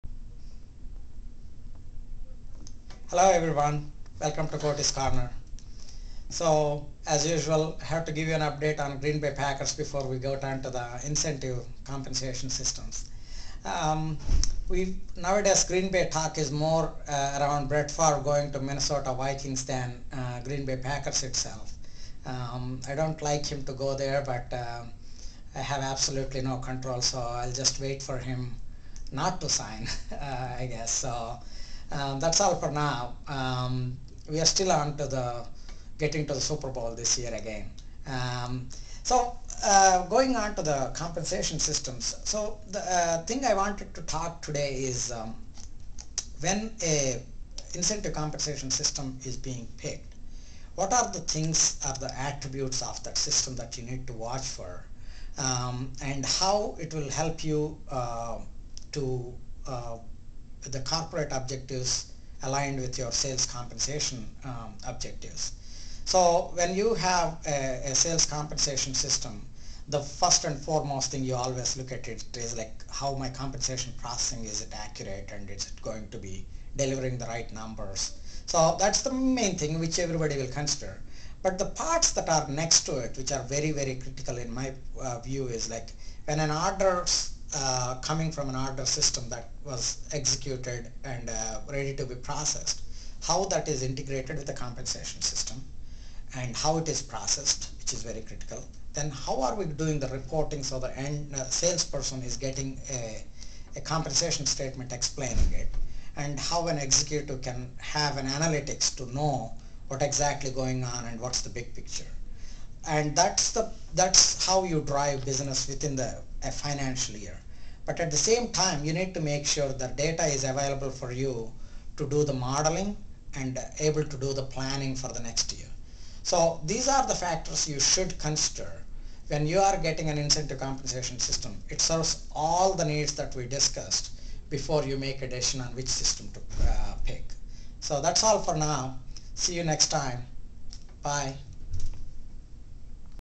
Callidus Software: Koti's Korner- June 16, 2009
In this segment of Kotis Korner, Callidus Softwares Koti Reddy reveals top factors to consider when evaluating the best incentive system for your company. Koti explains why companies must carefully choose an incentive system appropriate for their needs.